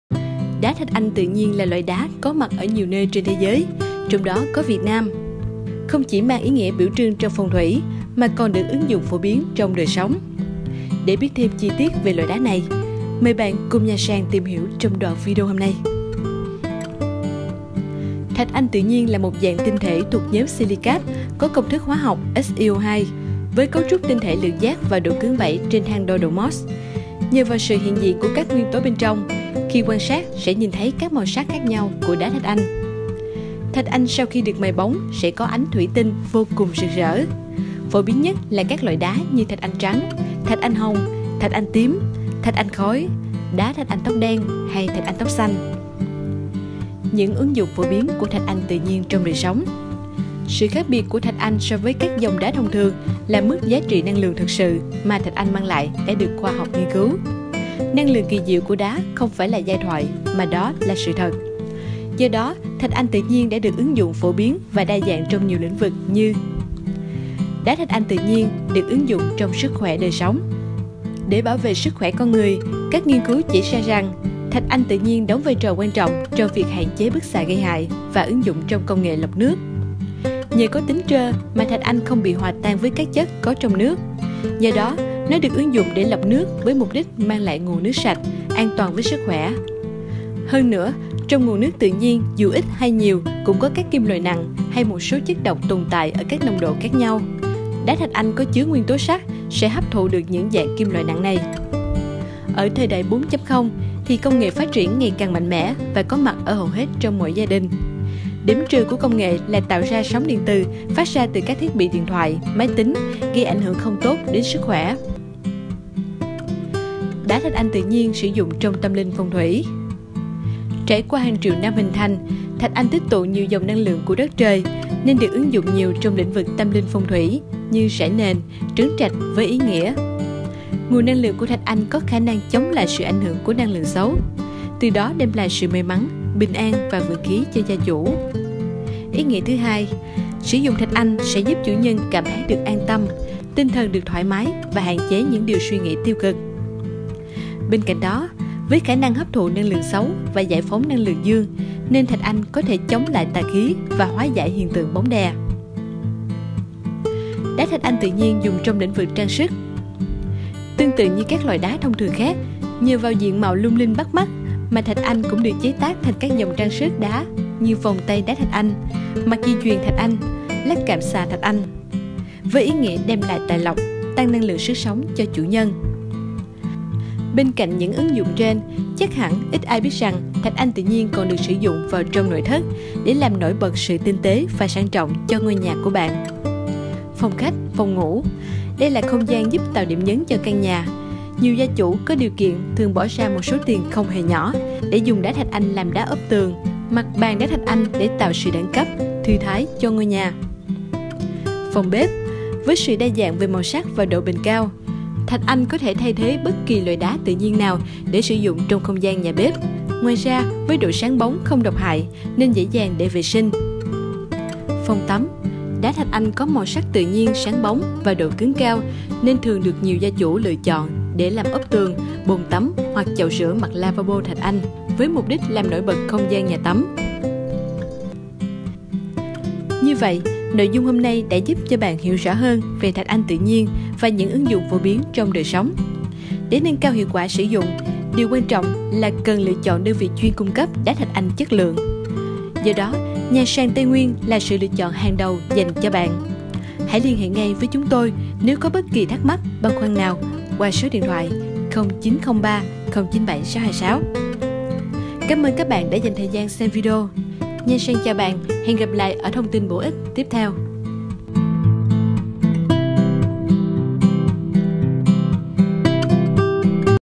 Đá Thạch Anh Tự Nhiên Và Những Ứng Dụng Phổ Biến Trong Đời Sống
Đá thạch anh tự nhiên là gì?
- Đá thạch anh tự nhiên là một dạng tinh thể thuộc nhóm Silicat có công thức hóa học là SiO2. Với cấu trúc tinh thể lượng giác và độ cứng 7 mohs. Nhờ vào sự hiện diện của các nguyên tố bên trong, khi quan sát sẽ nhìn thấy các màu sắc khác nhau của đá thạch anh.
- Đá thạch anh sau khi được mài bóng sẽ có ánh thủy tinh vô cùng rực rỡ. Phổ biến nhất là các loại đá như đá thạch anh trắng, đá thạch anh hồng, đá thạch anh tím, đá thạch anh khói, đá thạch anh tóc đen hay đá thạch anh tóc xanh.
Kho Đá Thạch Anh tự nhiên Việt Nam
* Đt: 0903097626